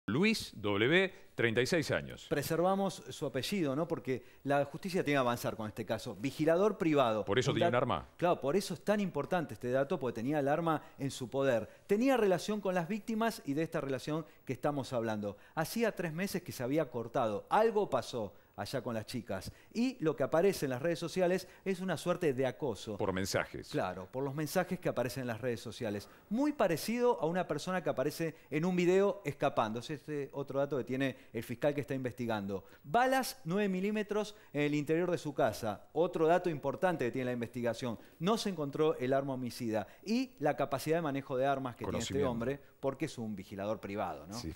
Fusiladas a la salida del boliche: un detenido -NOTI.20
Cuatro chicas menores de edad fueron víctimas de un brutal ataque de al menos 20 tiros en Florencio Varela. Dos jóvenes murieron mientras que las otras dos resultaron gravemente heridas. Por el hecho, hay un detenido. En este video, todos los detalles.